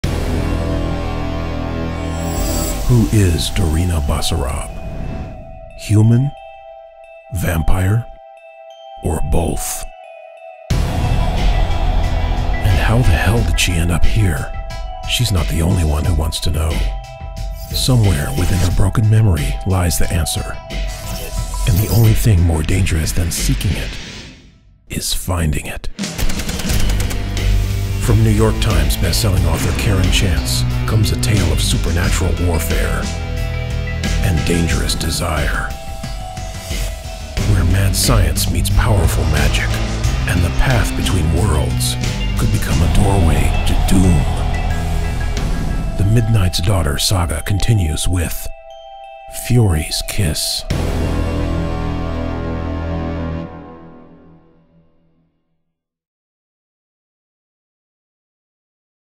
furys kiss
Book trailer for the fantasy novel Fury's Kiss by Karen Chance. This is the third novel in the Dorina Basarab urban fantasy series.